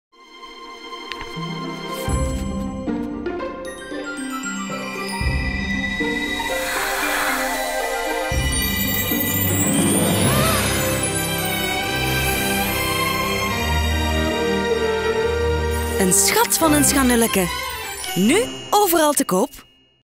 Een schat van een Schanulleke
De tv-spot voor het schattige Schanulleke-popje.